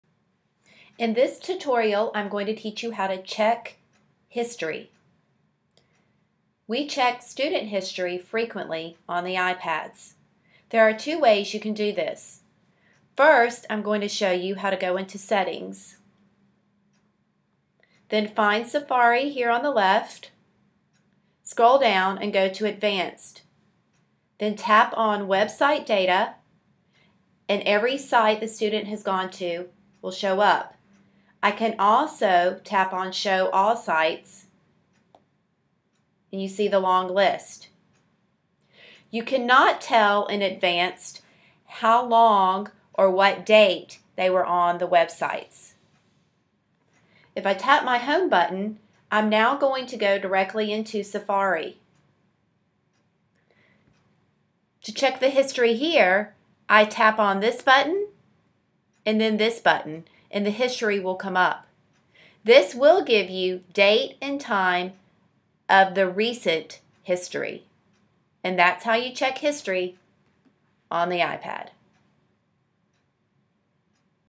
Checking History
Learn how to check history on the iPad.